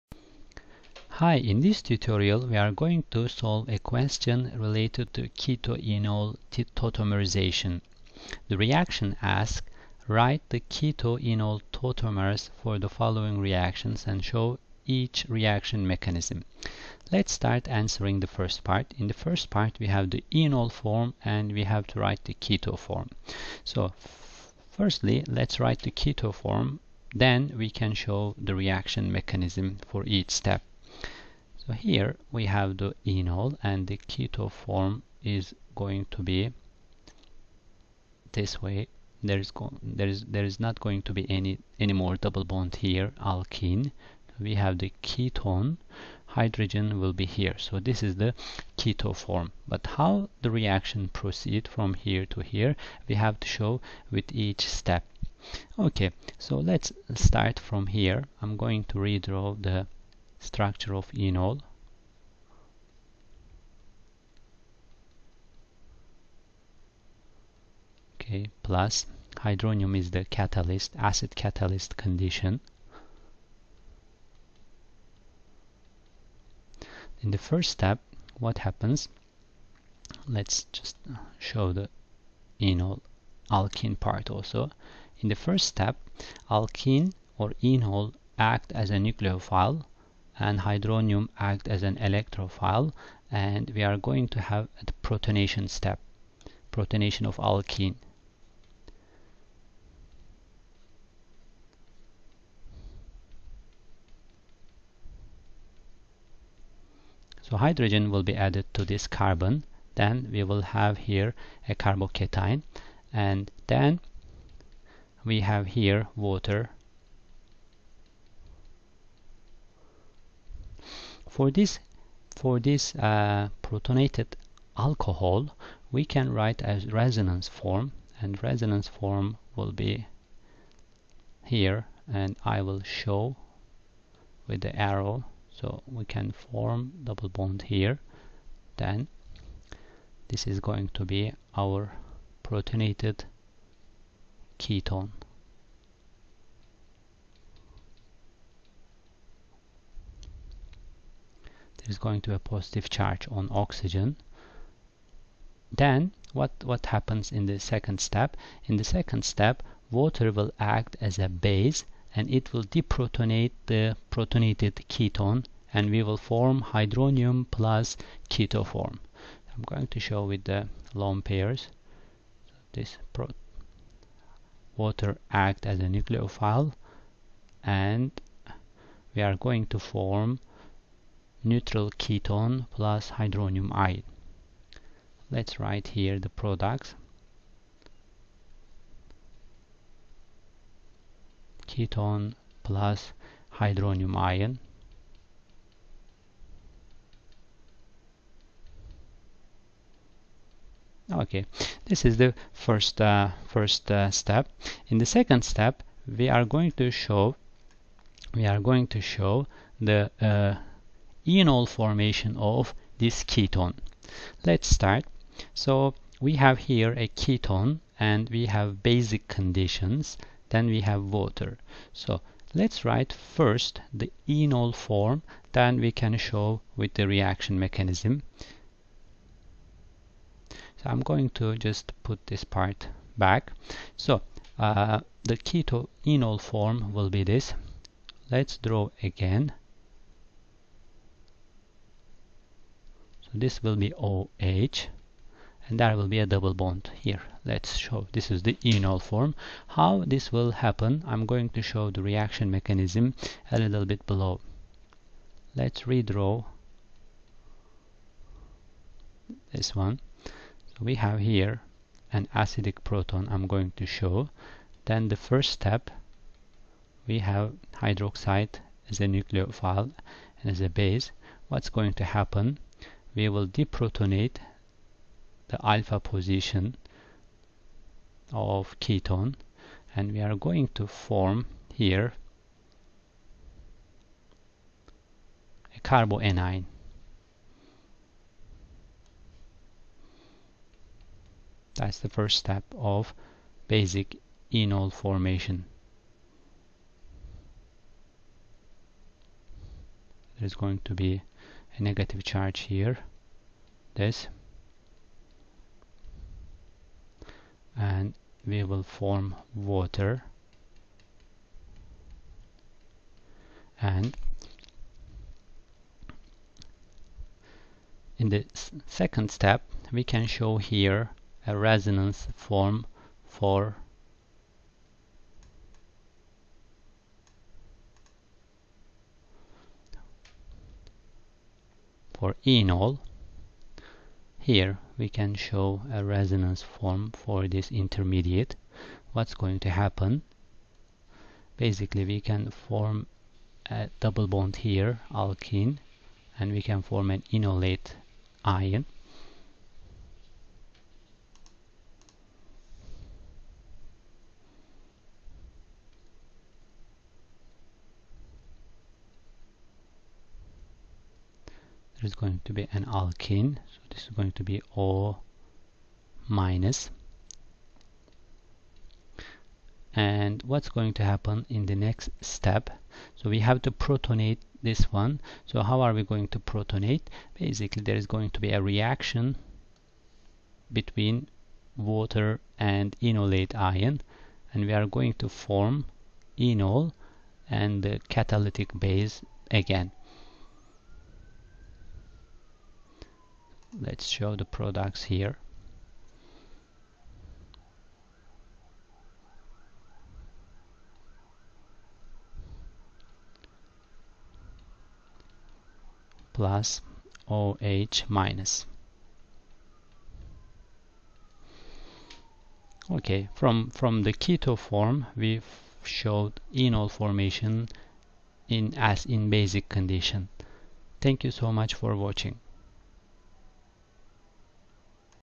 Keto–Enol Tautomerism
Mechanism of enol formation under both acid-catalyzed and base-catalyzed conditions